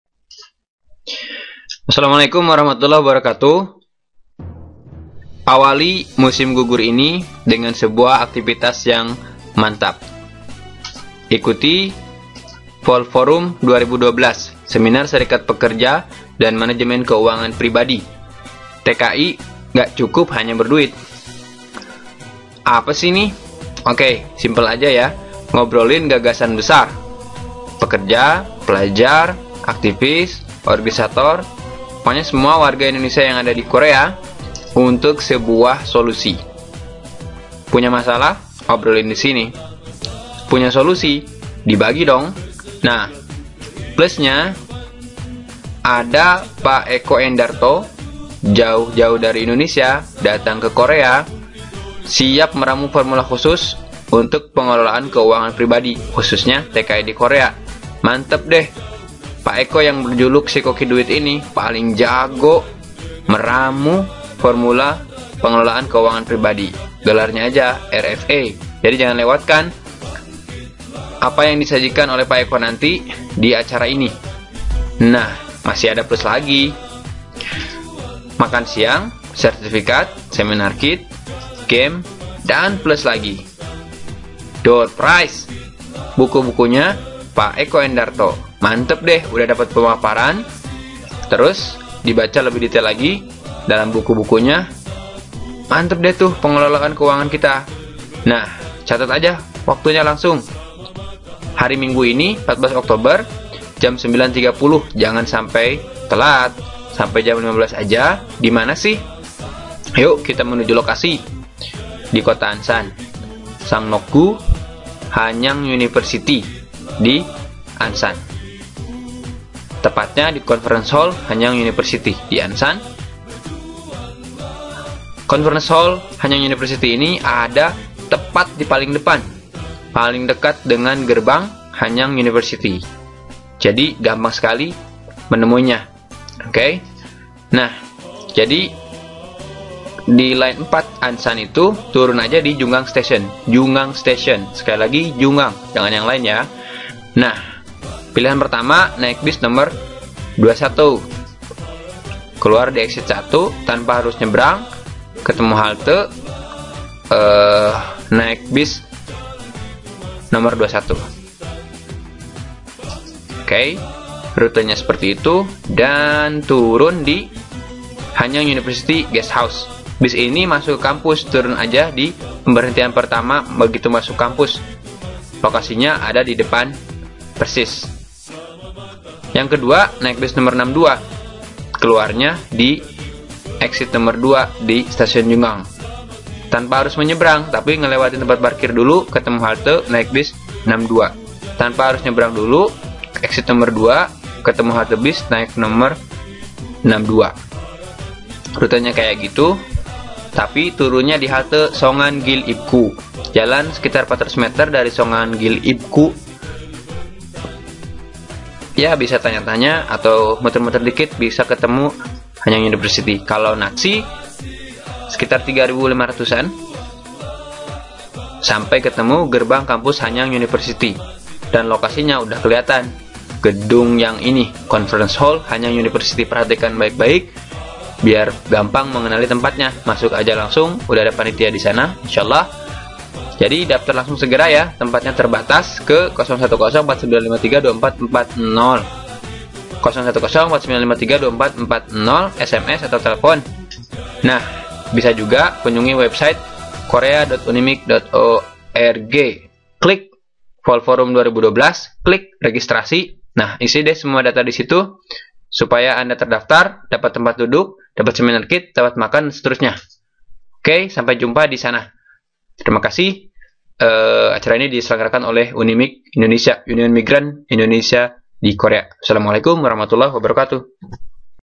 Promo Fall Forum 2012
Adalah sebuah forum silaturahim, edukasi dan diskusi interaktif yang diselenggarakan oleh UNIMIG Indonesia (Serikat Pekerja Migran Indonesia) di Korea pada musim gugur 2012 bagi kalangan terbatas dari TKI, Pelajar dan unsur warga Indonesia lain di seluruh Korea Selatan.